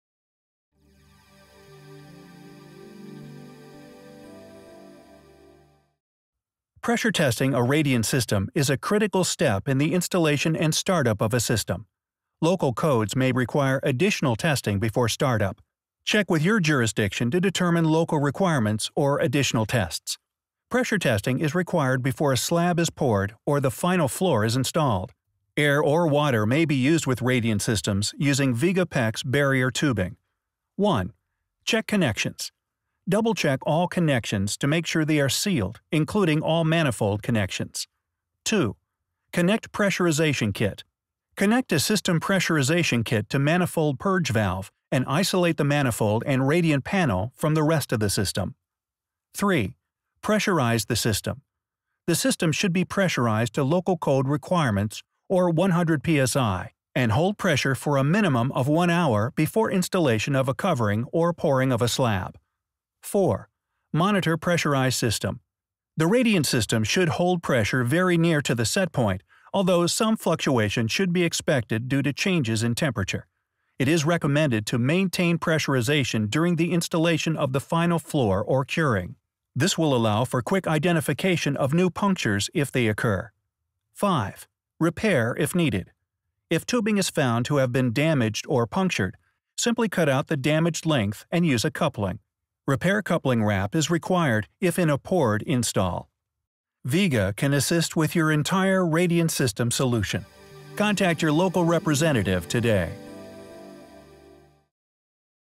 HOW TO PRESSURE TEST A RADIANT SYSTEM | Viega
Viega shows you how to pressure test a radiant system.